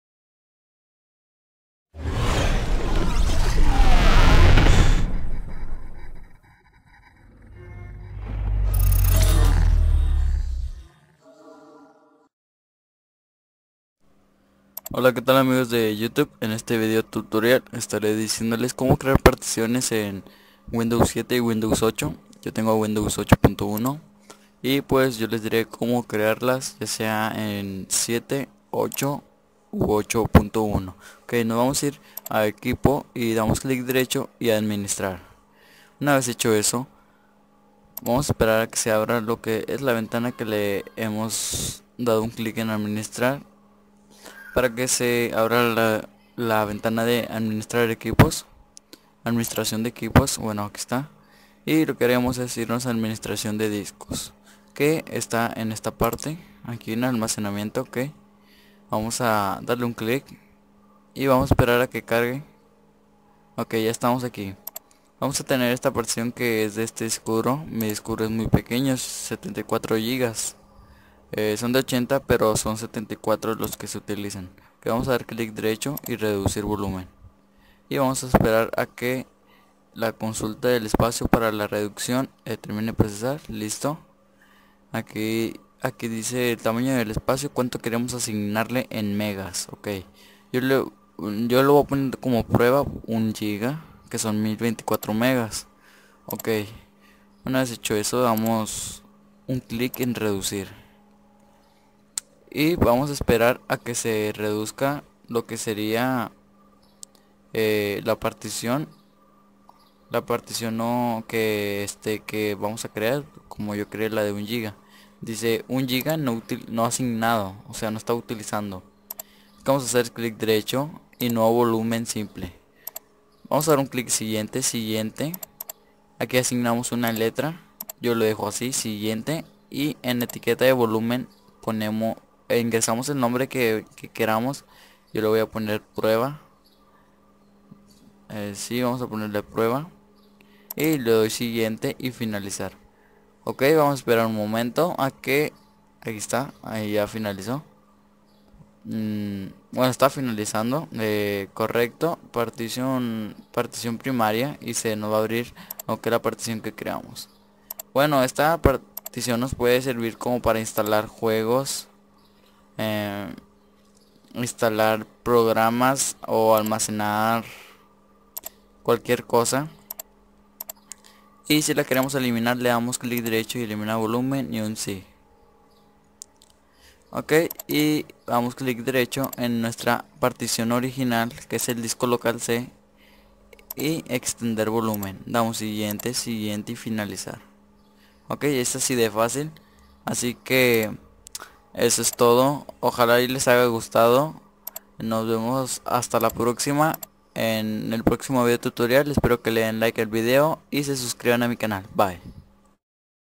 Como crear particiones en Windows 7, 8 y 8.1
Este es un pequeño tutorial dire como crear particiones en Windows.

¡Sigueme!